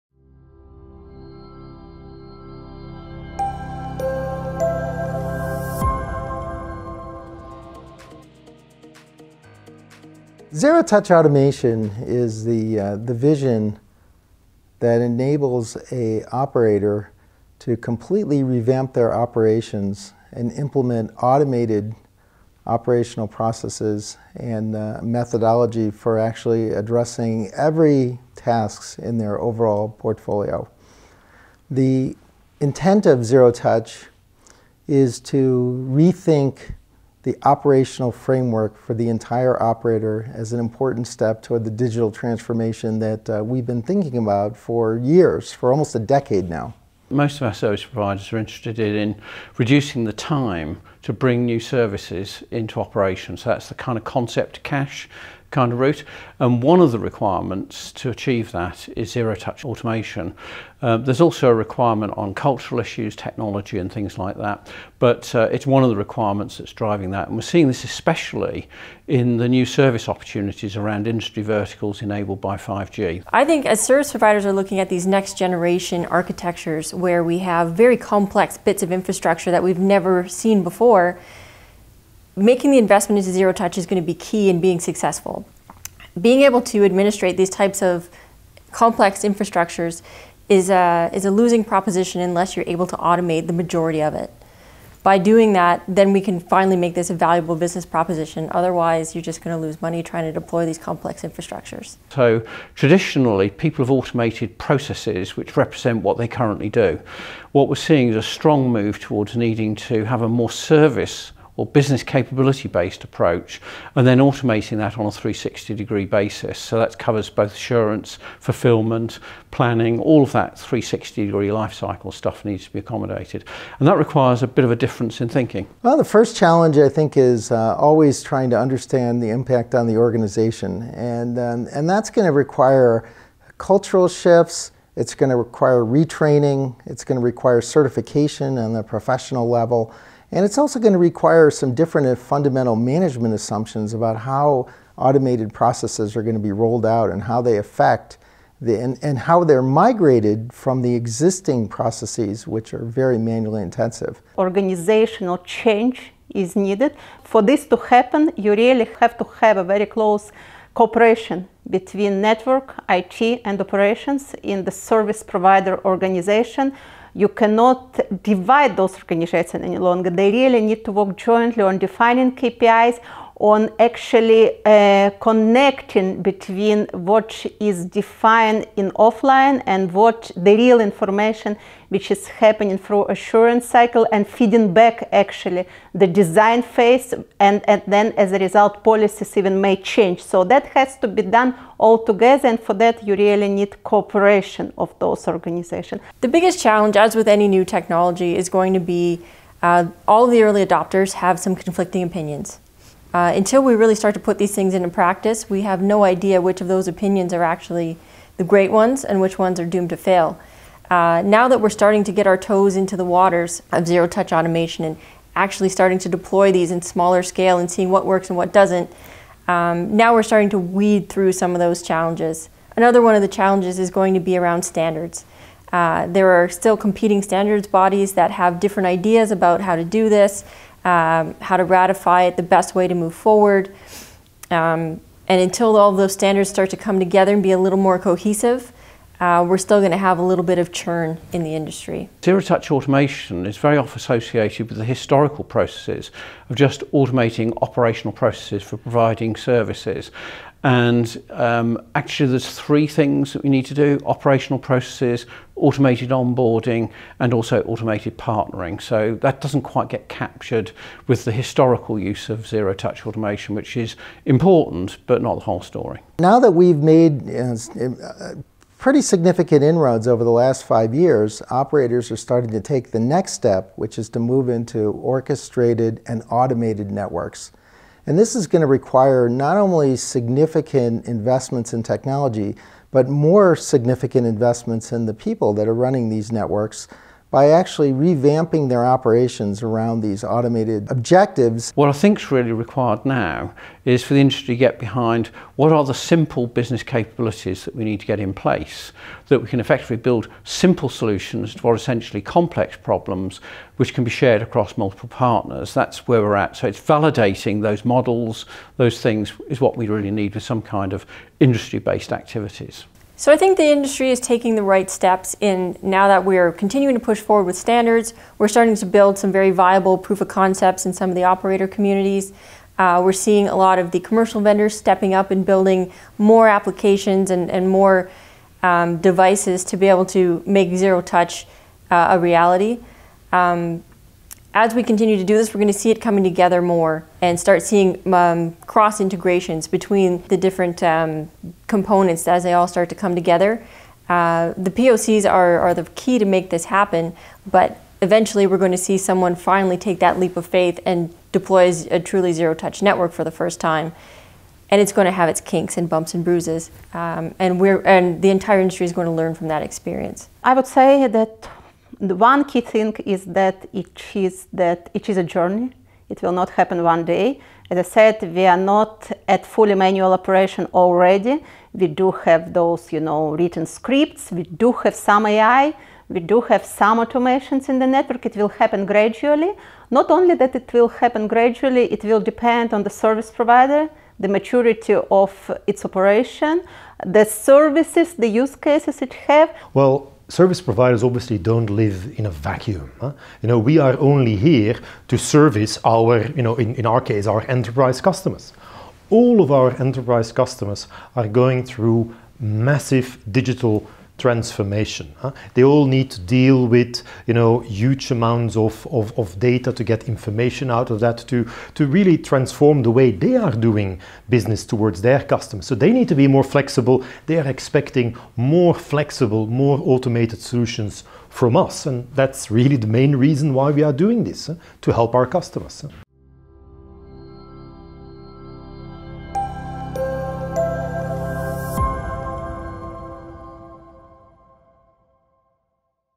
Achieving Zero Touch Automation
What is Zero Touch Automation and why should service providers be taking an active part in its development? CSPs are looking to reduce the time required to bring new services into operation, and one of the key elements necessary to achieve this is zero touch automation. The intent of zero touch is to rethink the operational framework for CSPs as an important step towards their ongoing digital transformation. But as well as technology changes, this will also require internal cultural changes, bringing together network IT and operations.

Featuring:
Alla Goldner, Director of Technology, Strategy and Standardization, Amdocs and Usecase Subcommittee Chair, ONAP
Marc Cohn, Virtualization Business Lead, Network Architects International
Dave Milham, Chief Architect, TM Forum
Vanessa Little, Director, Global NFV Ecosystem Architecture, VMware
Peter Konings, Head of Product Development, Europe, Verizon

Filmed at: ZTA Congress, Madrid, Spain, March 2019